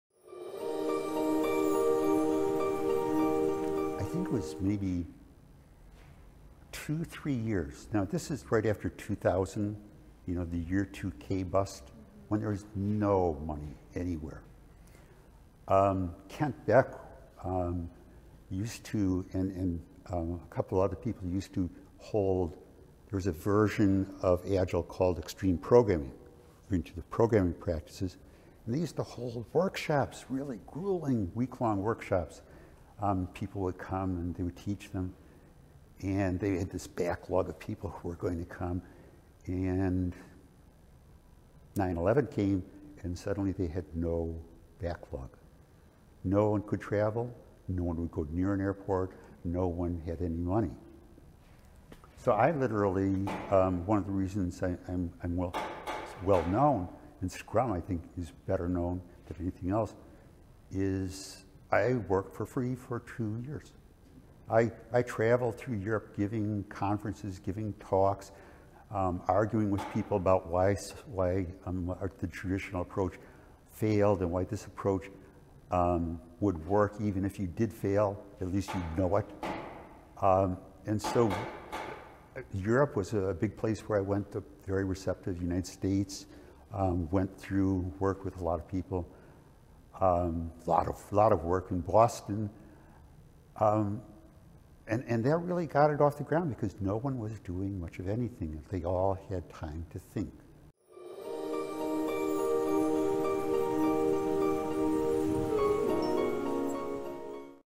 Hitting The Road for Agile
In an interview with the Boston Business Journal, Ken Schwaber, co-creator of Scrum and founder of Scrum. org discusses how he hit the road, traveling around the world in the early days of Agile to educate and enable those wanting to adopt it.